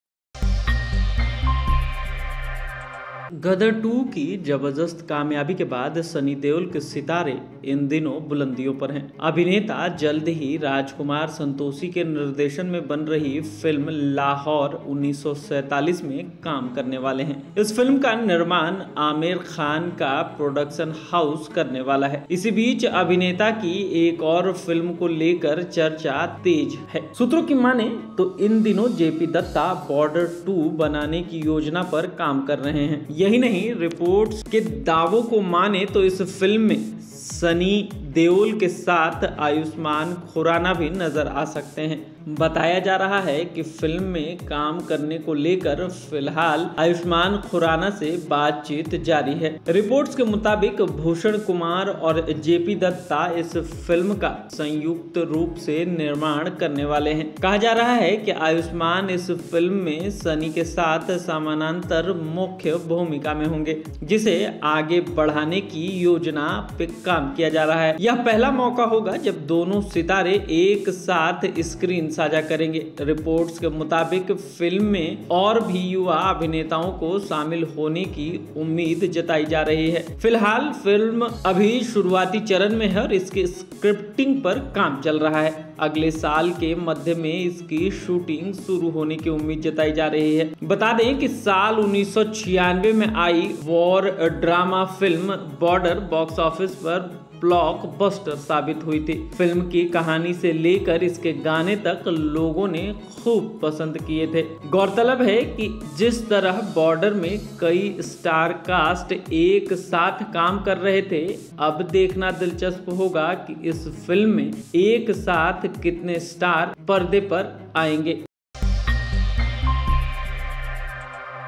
Border 2 Update | Release Date | सन्नी की अगली फिल्म बॉर्डर 2 पर चर्चा तेज कौन कौन होगा इस बार साथ
गदर 2 की जबर्दस्त कामयाबी के बाद सनी देओल के सितारे इन दिनों बुलंदियों पर हैं। अभिनेता जल्द ही राजकुमार संतोषी के निर्देशन में फिल्म लाहौर 1947 में काम करने जा रहे हैं। इस फिल्म का निर्माण आमिर खान का प्रोडक्शन हाउस करने वाला है। इस बीच अभिनेता की एक और फिल्म को लेकर चर्चा तेज हो गई है।

सूत्रों की माने तो इन दिनों जेपी दत्ता बॉर्डर 2 बनाने की योजना पर काम कर रहे हैं। यही नहीं, रिपोर्ट के दावों की मानें तो इस फिल्म में सनी के साथ आयुष्मान खुराना भी नजर आ सकते हैं। बताया जा रहा है कि फिल्म में काम करने को लेकर फिलहाल उनसे बातचीत जारी है।

After the tremendous success of Gadar 2, Sunny Deol's star is at its peak these days. The actor is soon going to work in the film Lahore 1947 under the direction of Rajkumar Santoshi. Aamir Khan's production house is going to produce this film. Meanwhile, the discussion about another film of the actor has intensified.

If sources are to be believed, these days JP Dutta is working on a plan to make Border 2.